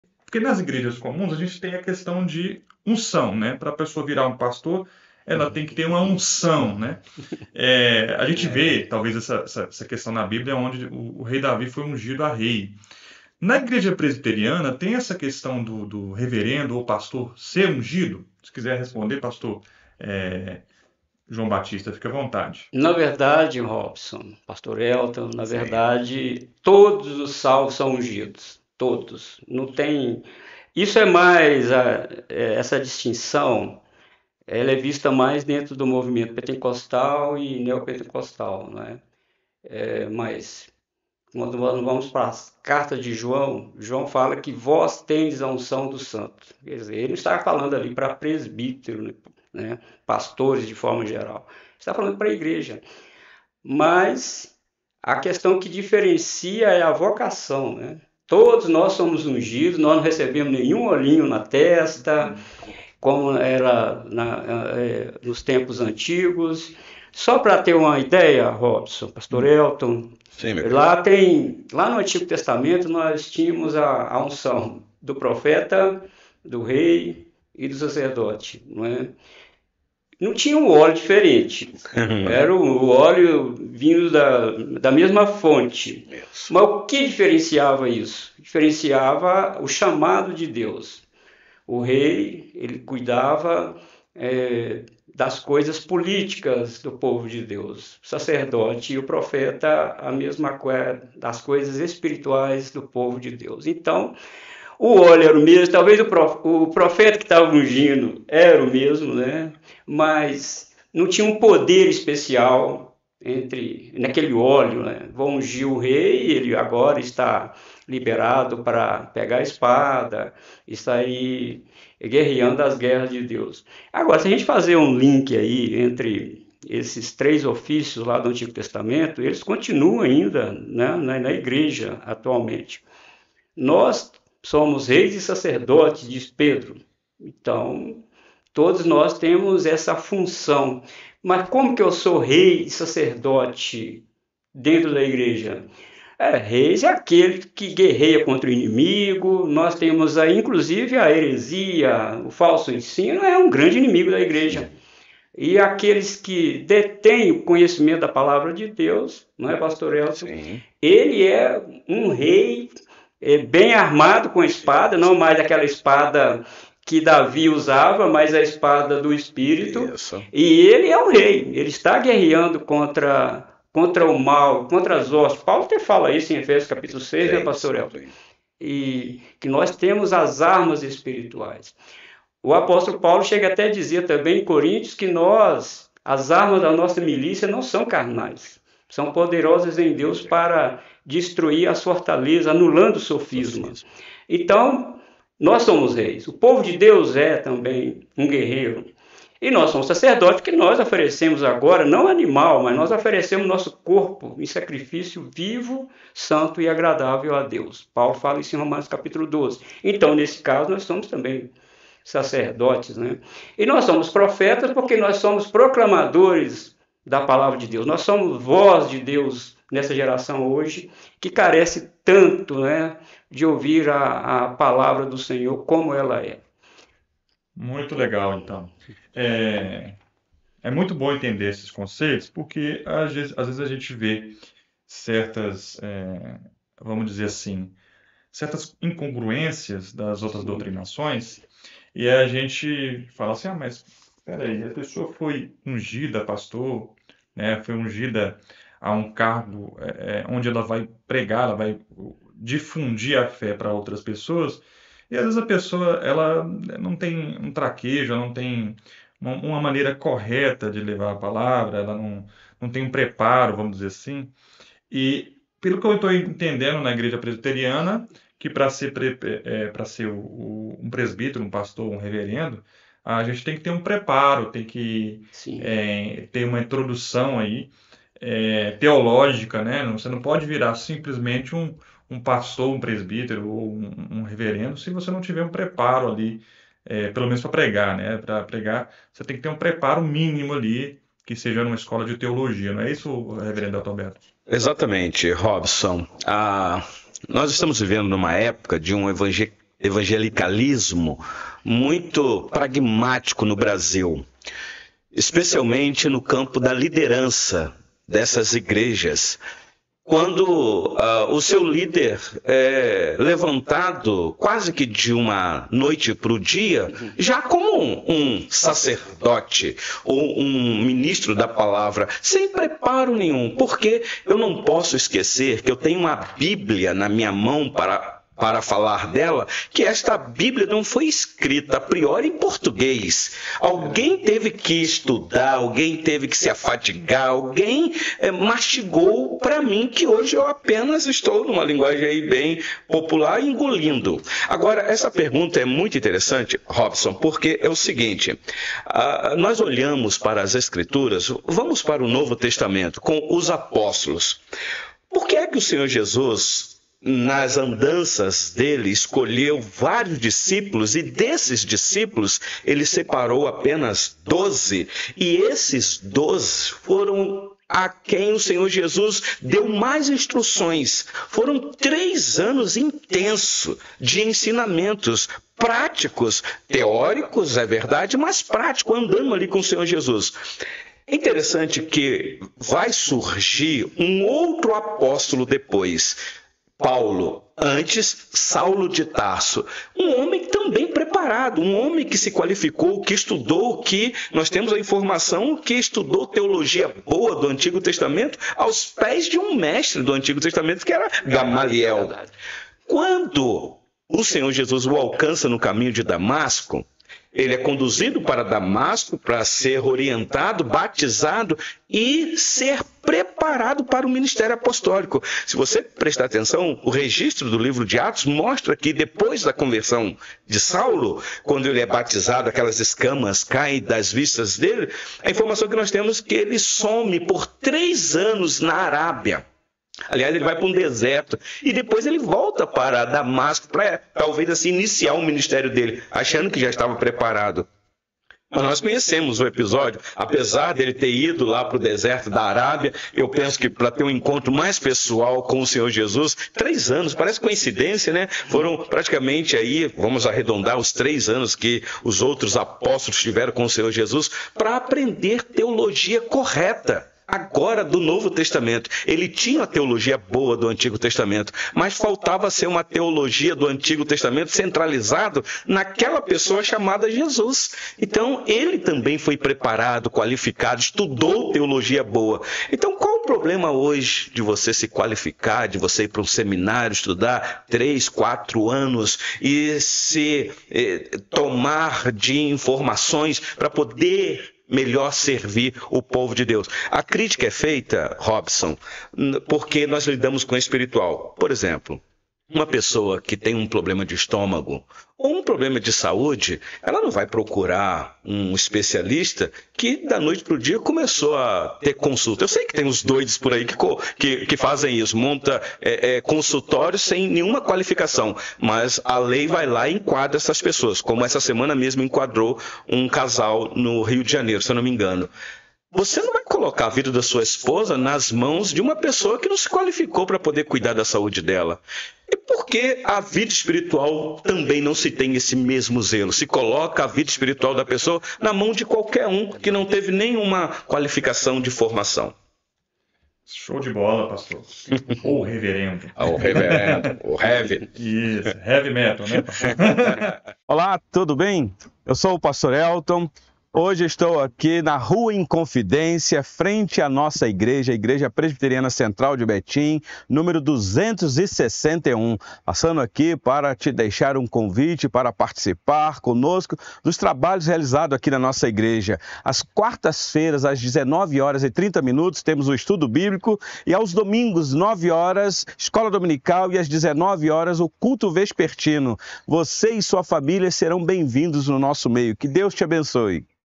Existe Unção para Líderes? | Visão Reformada na Igreja Presbiteriana 🎙️
🎙️✨ Em um episódio esclarecedor do Presbcast, exploramos o significado da unção a líderes e pastores na Igreja Presbiteriana, sob a visão reformada. 🕊️💡 Junte-se aos Reverendos Elton Roberto e João Batista enquanto mergulham nas nuances dessa prática na fé reformada, abordando princípios bíblicos e tradições presbiterianas.

Descubra como a unção se entrelaça com o preparo de líderes, influenciando a liderança cristã na Igreja Presbiteriana. 📖🙏 Este episódio proporcionará insights valiosos para aqueles que buscam compreender melhor a abordagem reformada em relação a esse tema crucial.